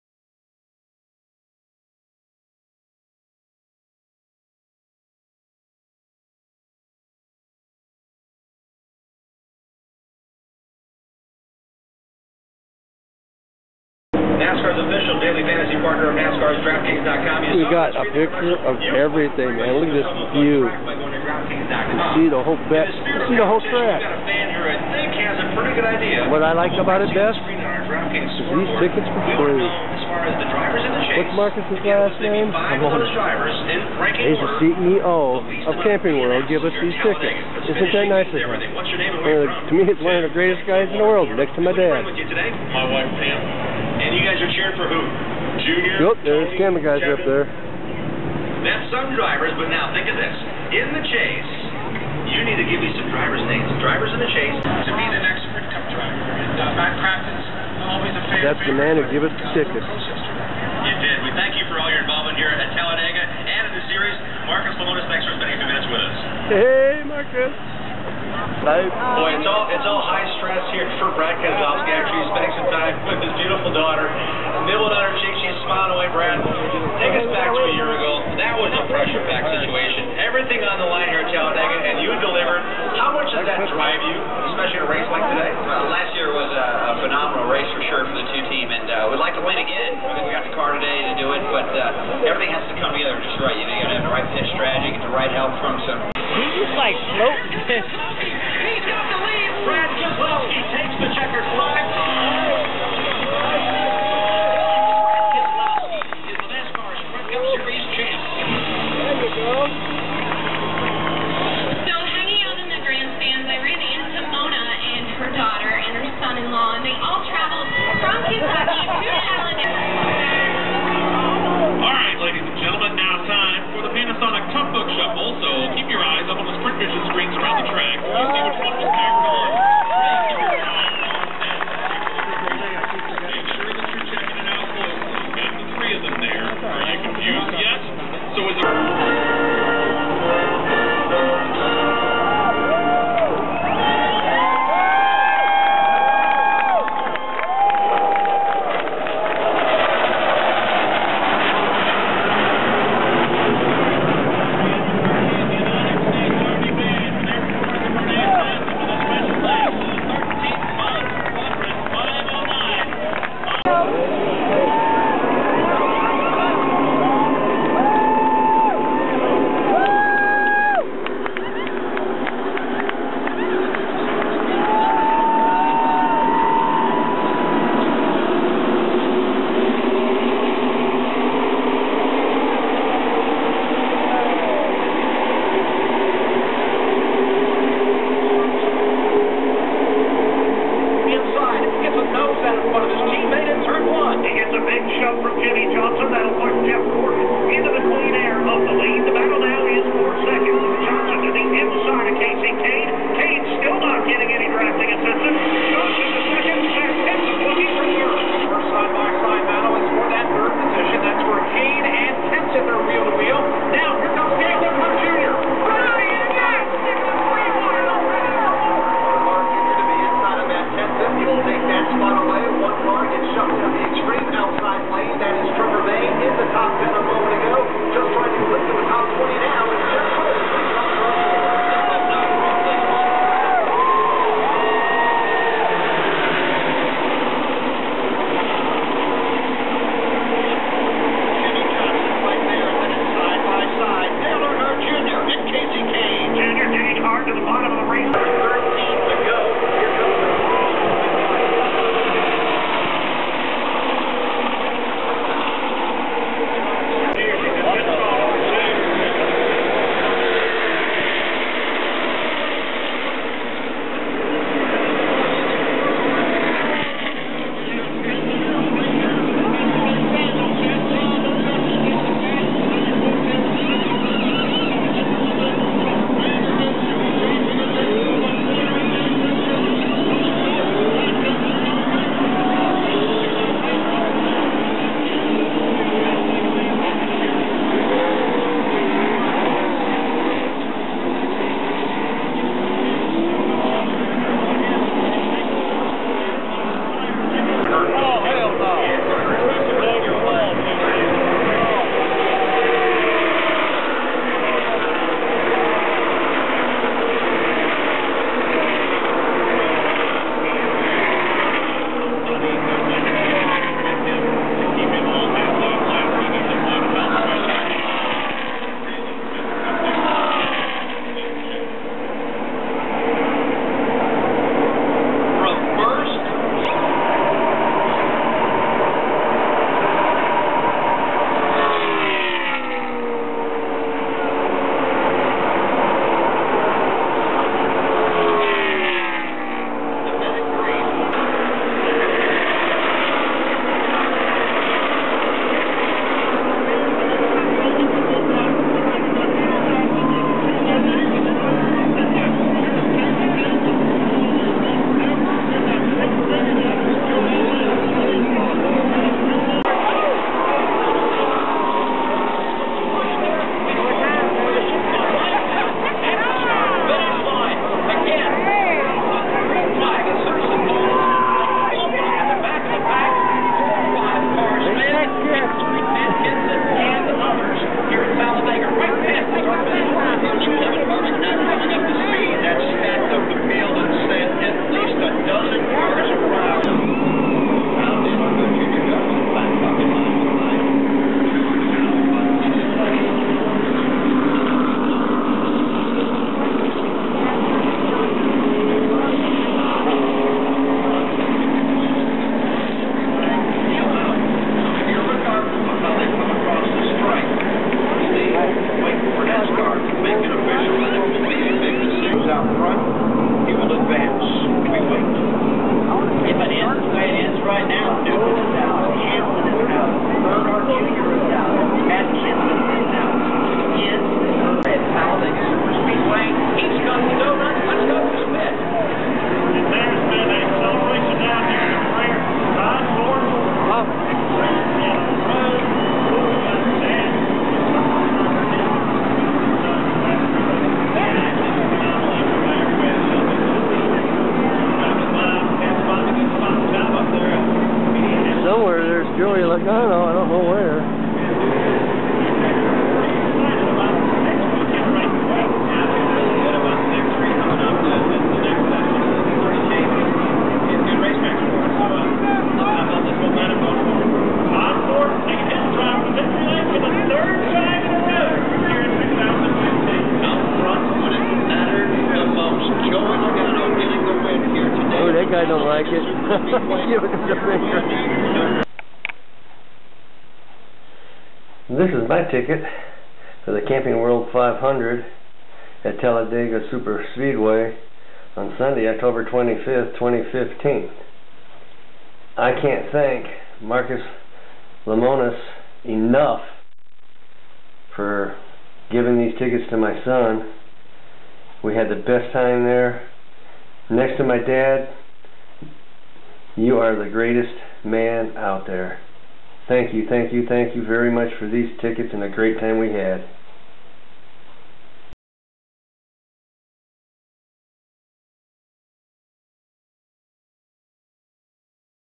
2015 Camping World 500
This is a video of some small clips I put together from The 2015 Camping World 500 race at Talladega.. I can't thank Mr. Marcus Lemonis enough for the FREE tickets that he gave my son for this race.. Thank you VERY much..